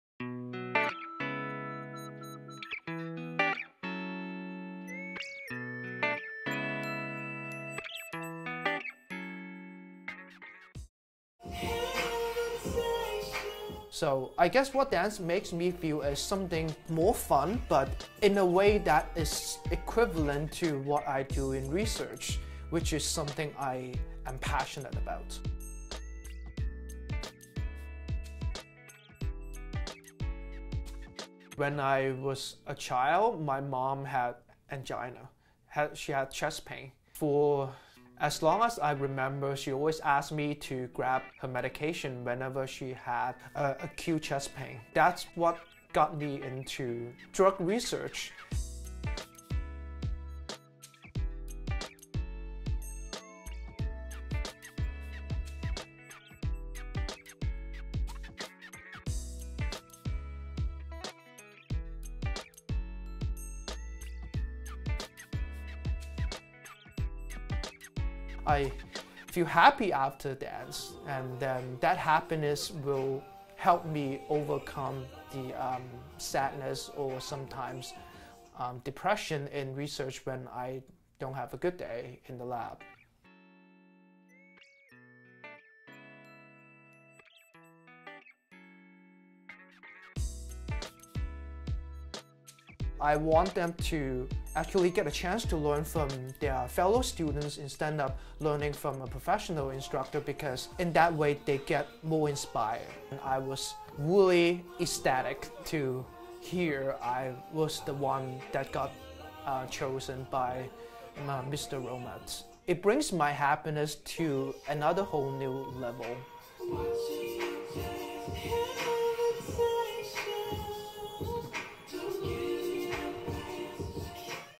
Kangbin Zhou: Dance and Discovery
By day, Kangbin Zhou researches what makes nitroglycerin — a common treatment for angina — effective. By night, he can be found tearing up the dance floor. Zhou is the first recipient of the Dr. Malle Jurima-Romet Award in Pharmacology and Toxicology, which recognizes academic excellence and involvement in the arts.